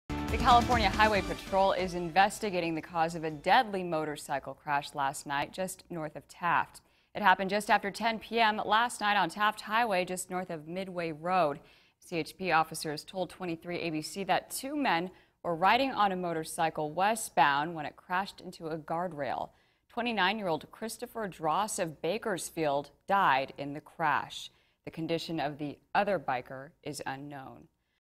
Bakersfield man killed in motorcycle crash north of Taft
29-year-old Christopher Dross of Bakersfield died in the crash.